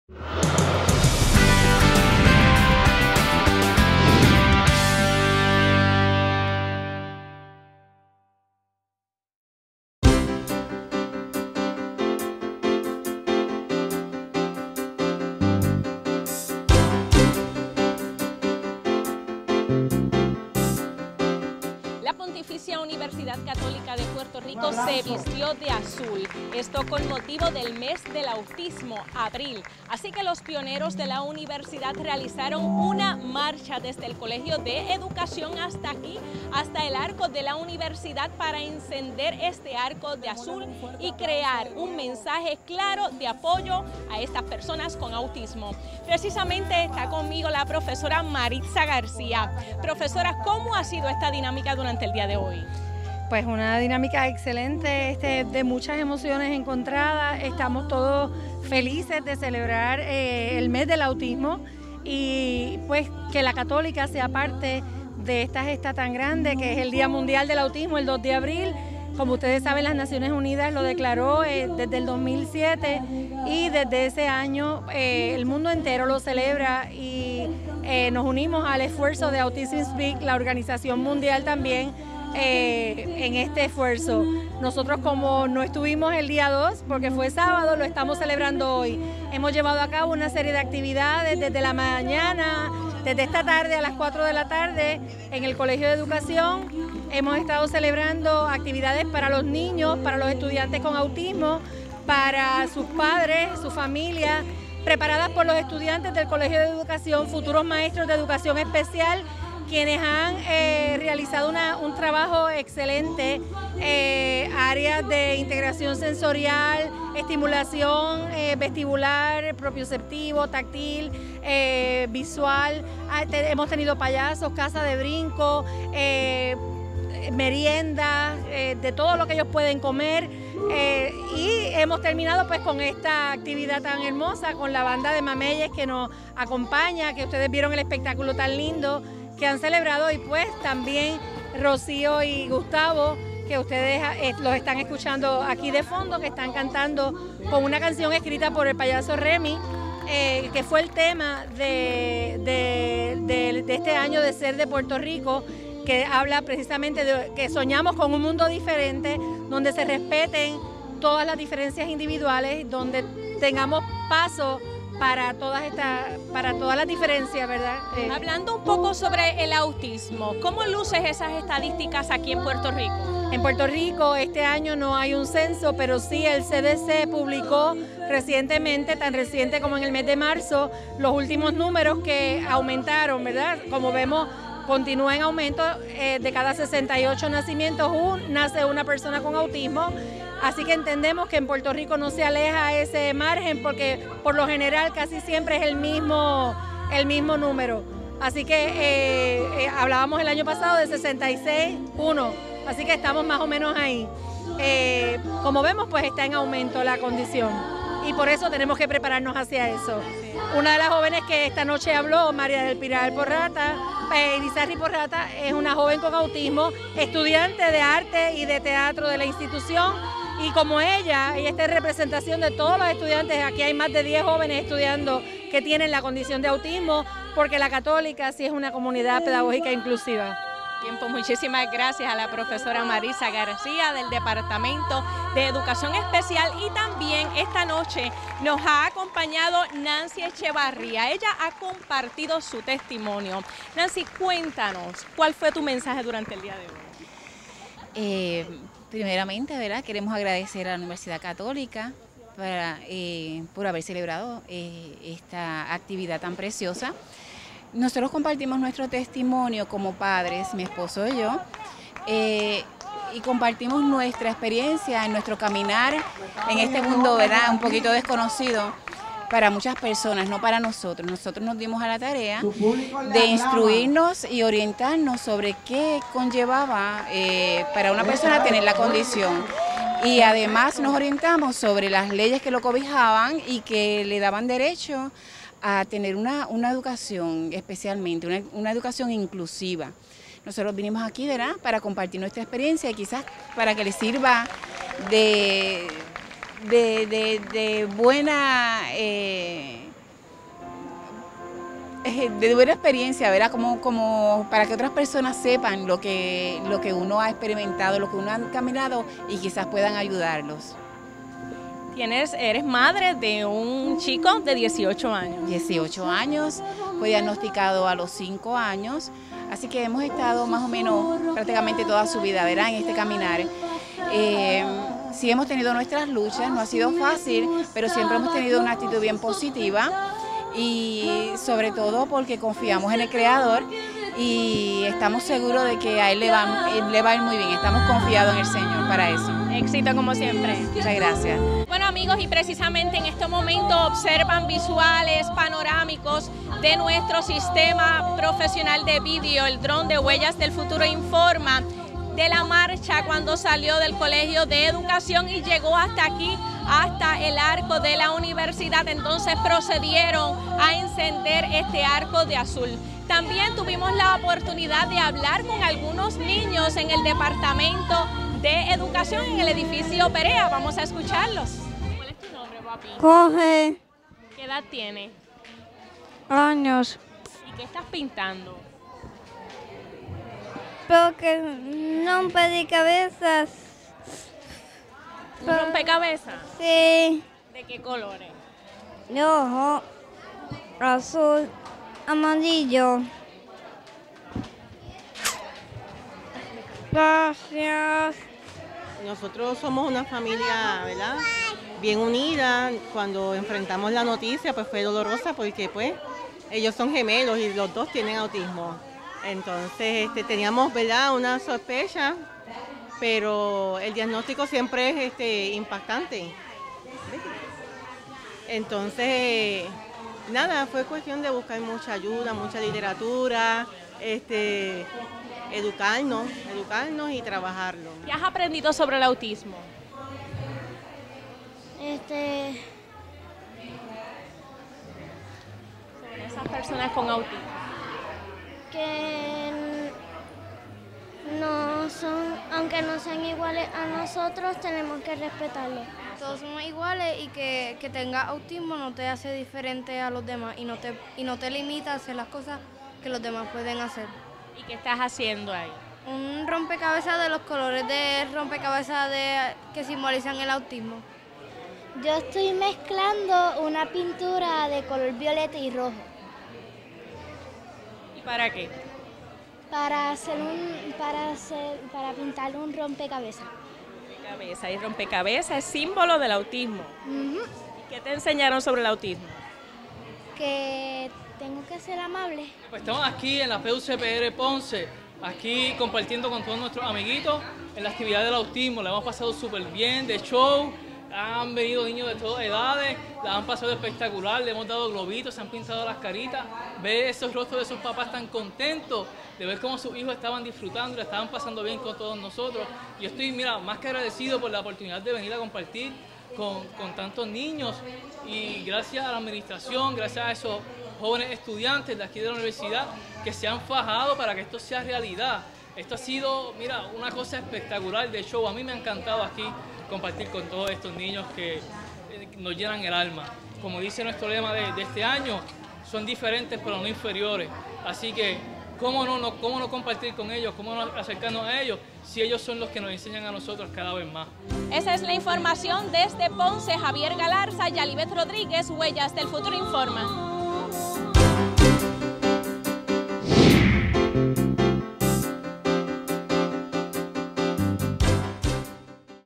Light it up blue
Durante todo el mes de abril se iluminan, alrededor del mundo, edificios de color azul en apoyo al autismo.  Unidos para concienciar a la comunidad sobre esta condición y romper mitos, el arco de La Católica, símbolo emblemático de la institución, se iluminó de azul el pasado 7 de abril de 2016 bajo el lema “Diferente, pero no inferior” de Temple Grandin, una mujer autista que revolucionó las prácticas del manejo de animales en ranchos ganaderos y mataderos.